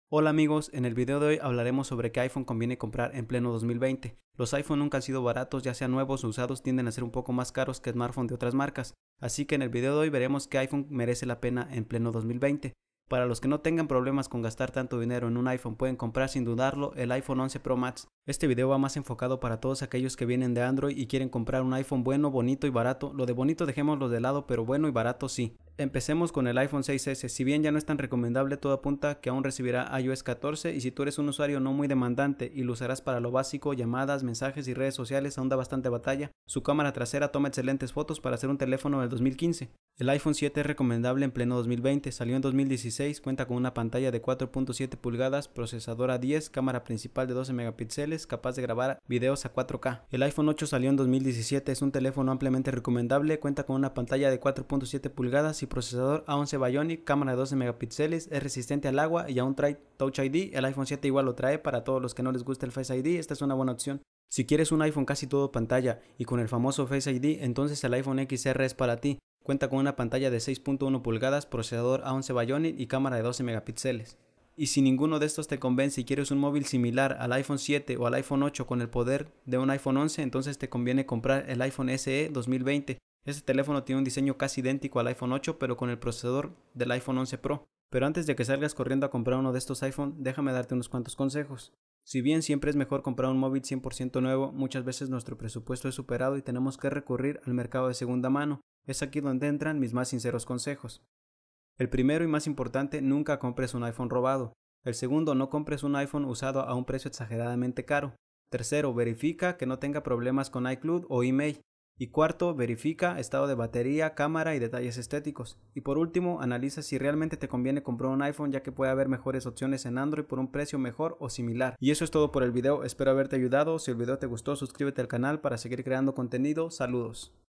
QUE IPHONE VALE LA PENA COMPRAR EN (2020)
Mira este vídeo antes de comprar un iphone en 2020. Estos son los modelos de iphone que aún vale la pena comprar, el iphone 6s sigue siendo recomendable según el uso que le vayamos ha dar.